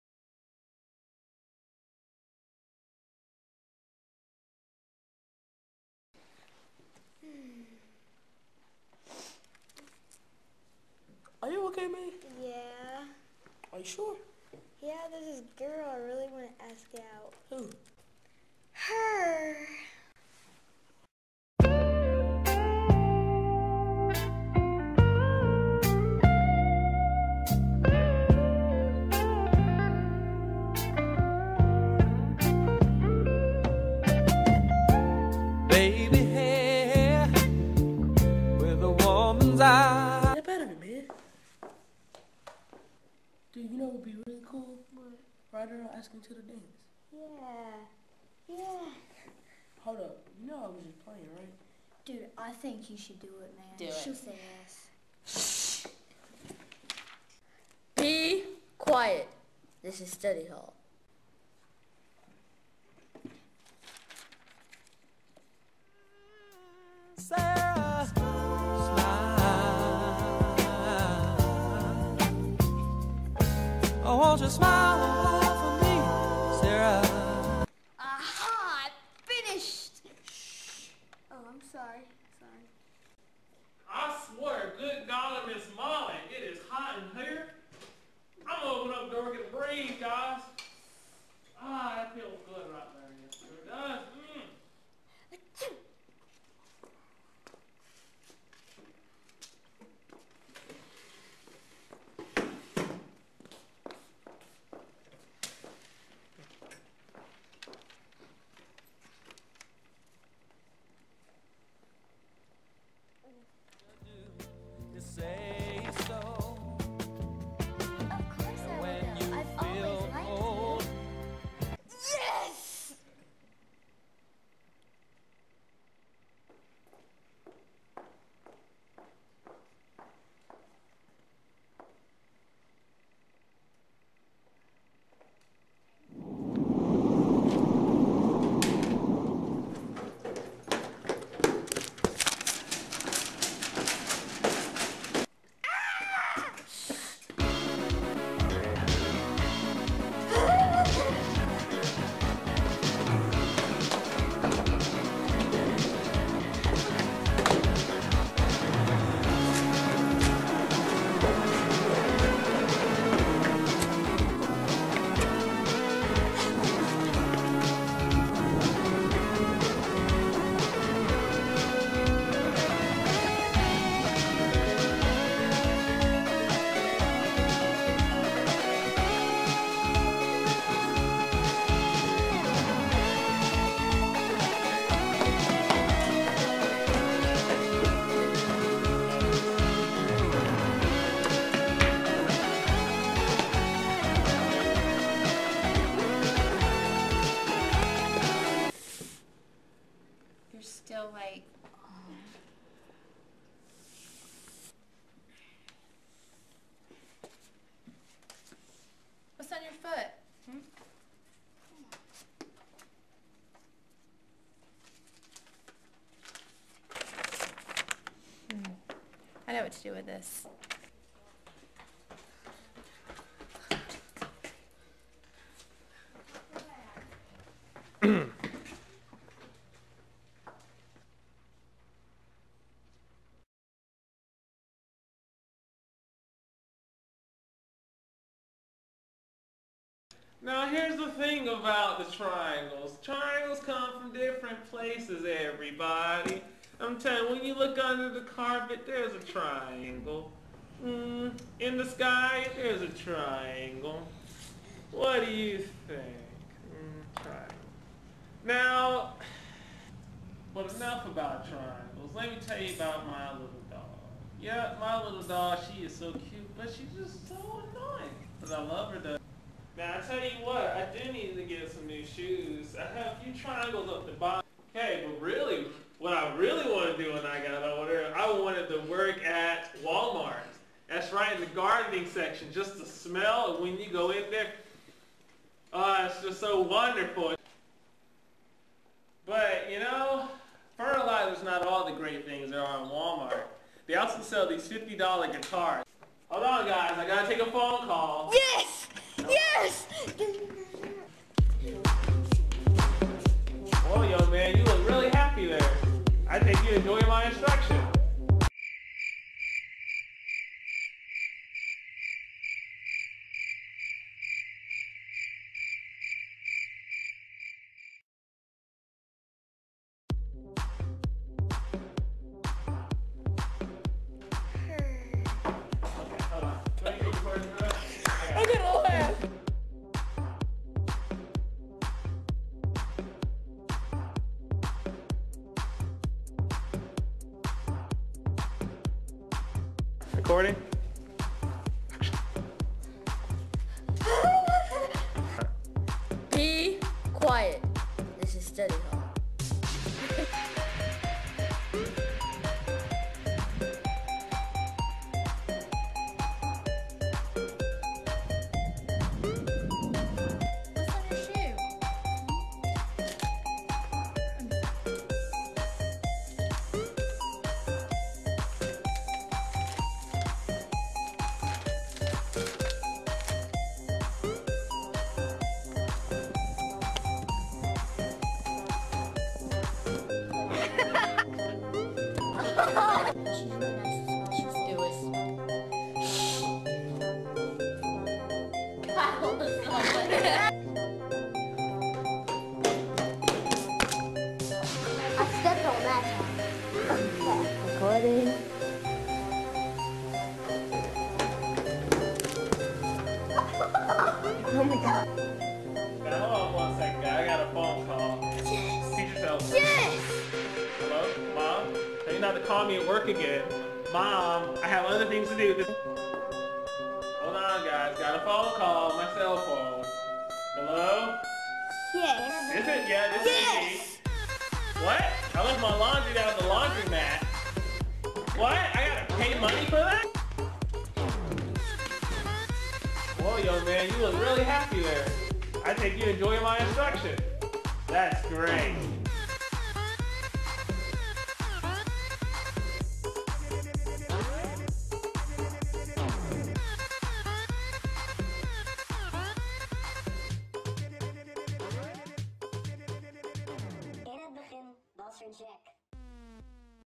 Bears News Network and Global Student Council presents A Letter For Sara
A young boy has a crush on a girl. After trying to deliver the note, it gets blown out the door and stuck on someone's shoe.  He is forced to chased down the note throughout school.